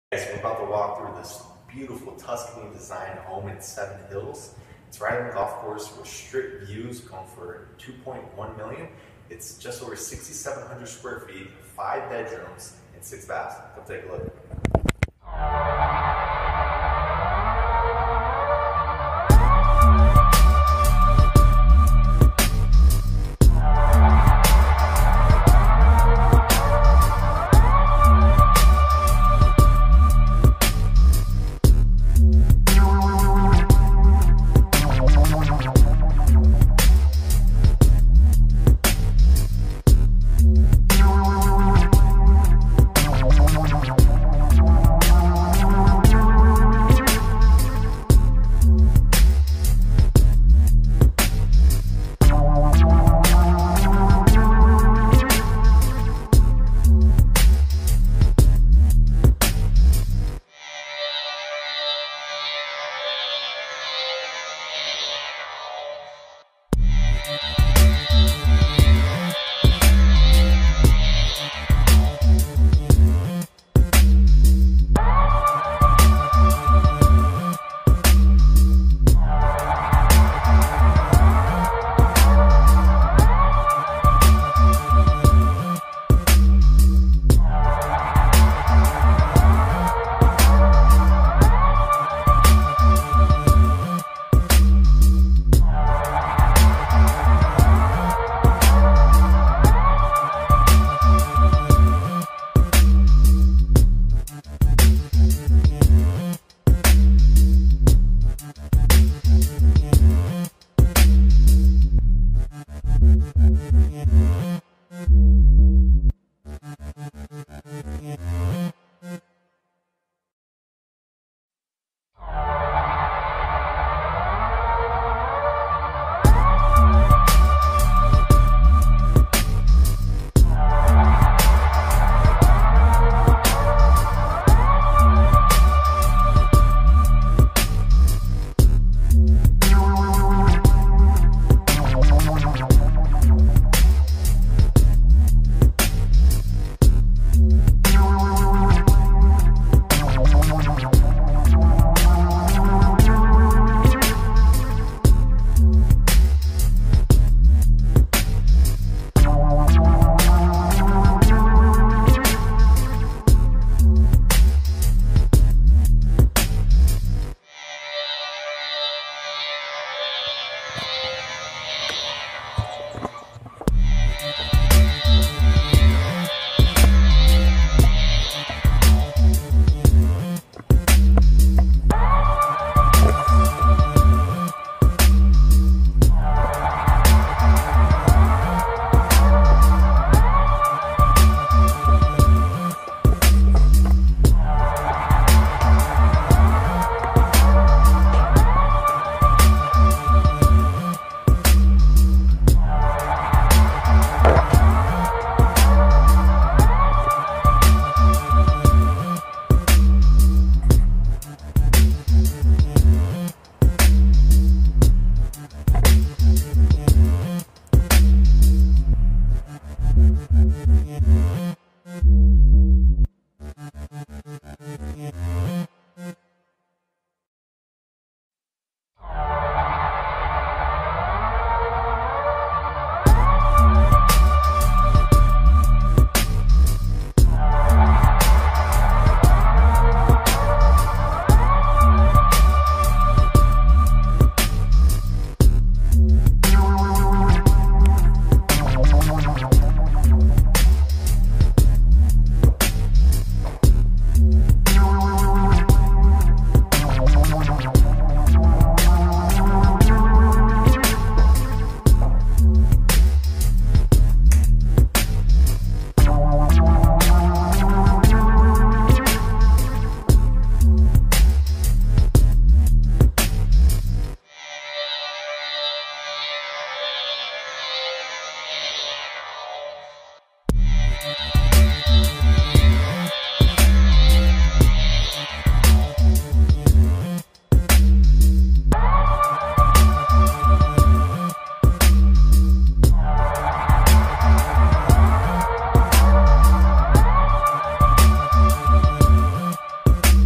Stunning Home with Strip Views
Beautiful Tuscany designed home in Seven Hills.. this is a 6700 sq ft home, golf course views, unprecedented strip views, 5 beds, 6 baths all for around $2.1 million.

Joseph Walters
Simply Vegas Real Estate
Diamond Estates - The Walters Team
NVRE S.172319